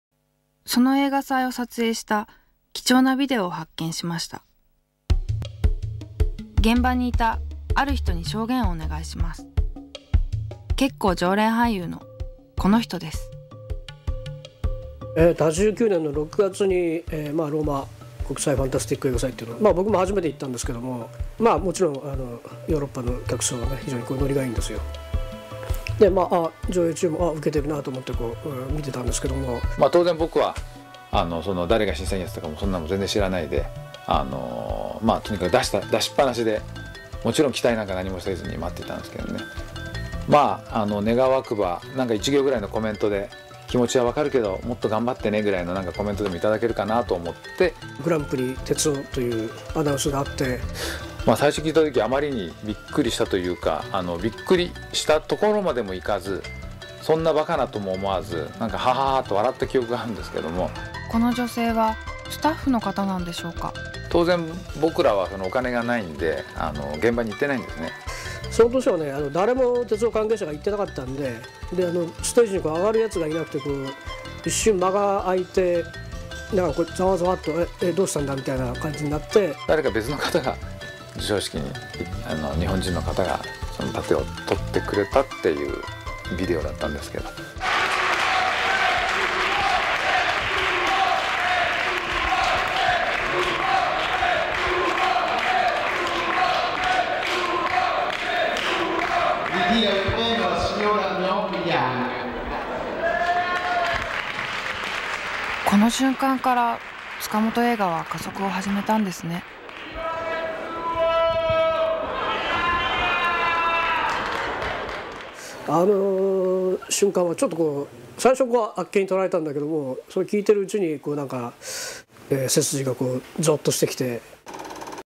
TETSUO di S.Tsukamoto premiato al FANTAFESTIVAL di Roma nel 1989.
Premiazione di Tetsuo di Shinya Tsukamoto al Fantafestival di Roma 1989.
Tetsuo: The Iron Man - Fantafestival (Italy) - Grand Prize 1989.